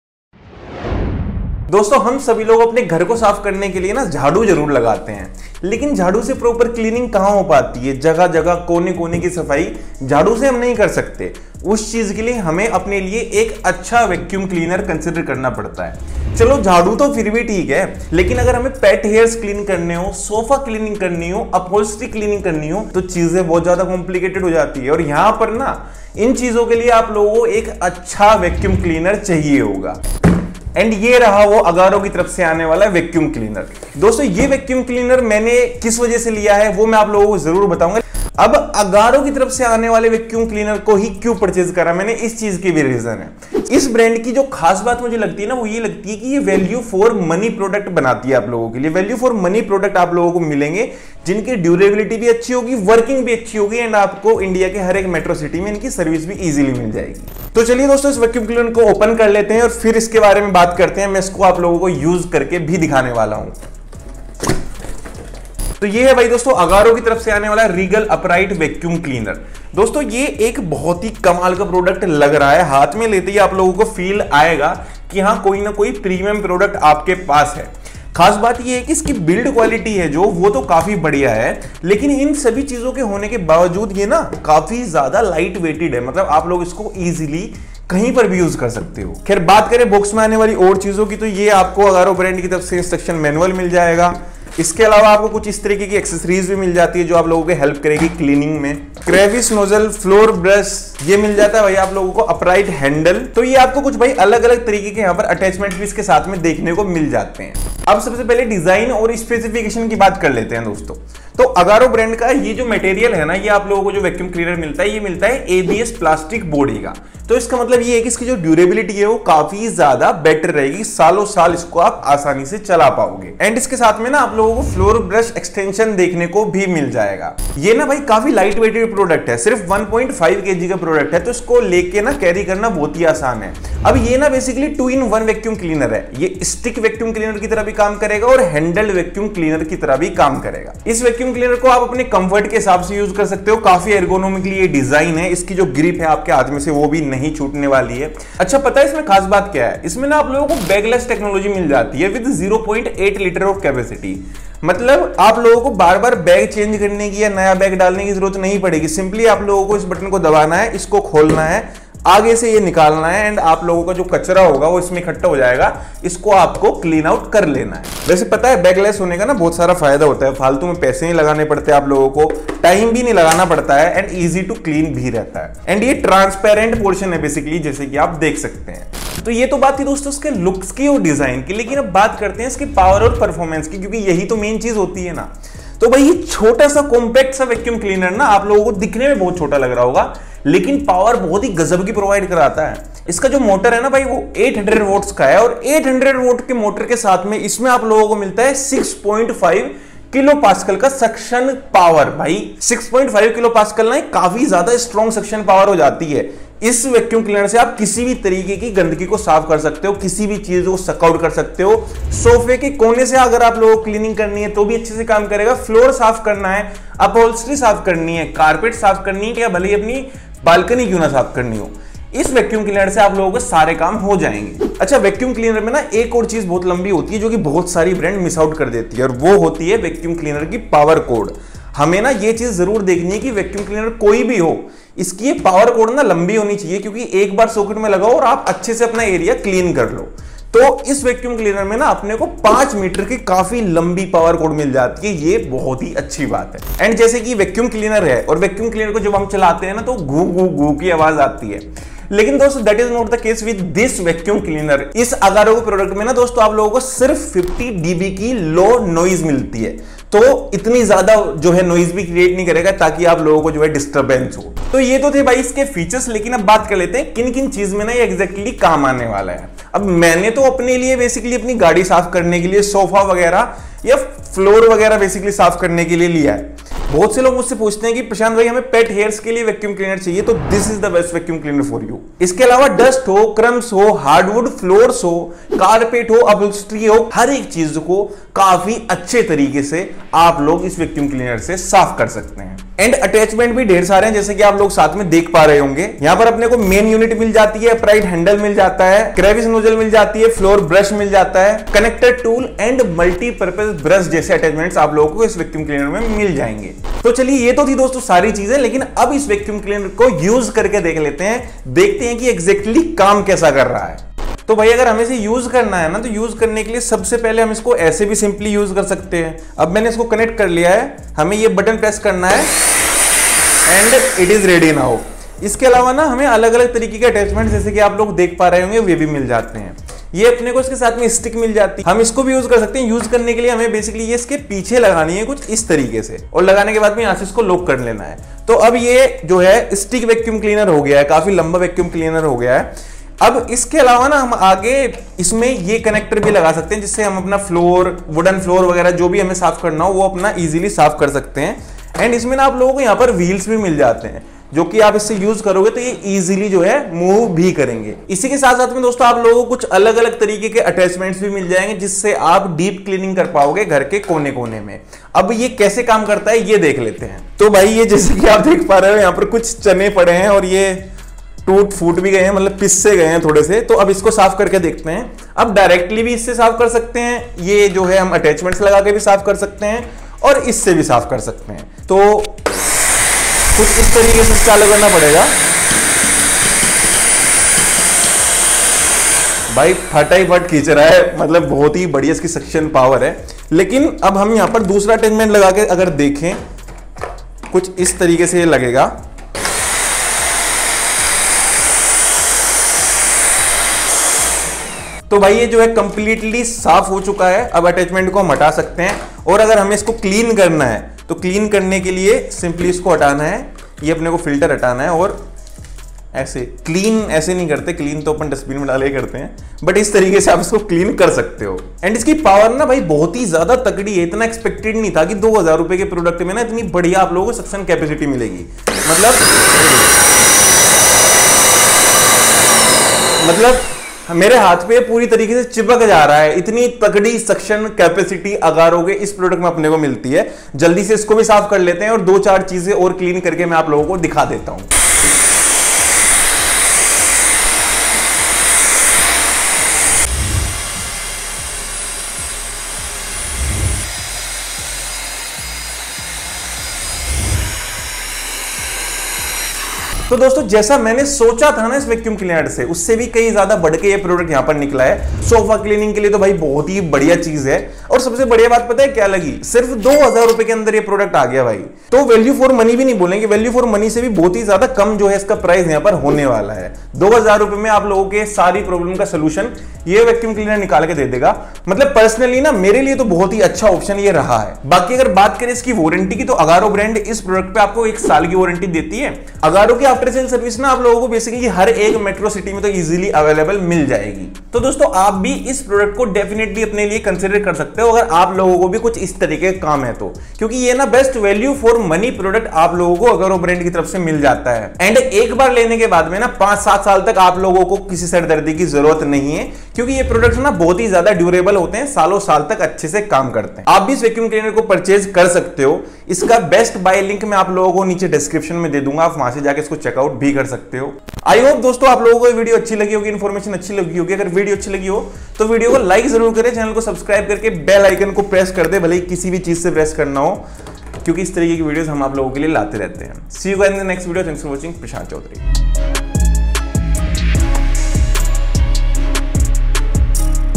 UNBOXING AGARO REGAL PLUS VACUUM CLEANER - Is Agaro Regal Plus Vacuum Cleaner Best in Budget ?
Checkout This Vacuum Cleaner From Here -

AGARO Regal Handheld Vacuum Cleaner Without Floor Brush Extension

**With Floor Brush Extension

Do you want a vacuum cleaner that is good value for your money? Check out the Agaro Regal Plus vacuum cleaner! This powerful cleaner is perfect for both small and large spaces, and it comes with a great warranty. In this video, we'll unbox the Agaro Regal Plus vacuum cleaner and discuss its features.

Does the Agaro Regal Plus vacuum cleaner have all the features you're looking for?

Prashant Chaudhary
Product Bazar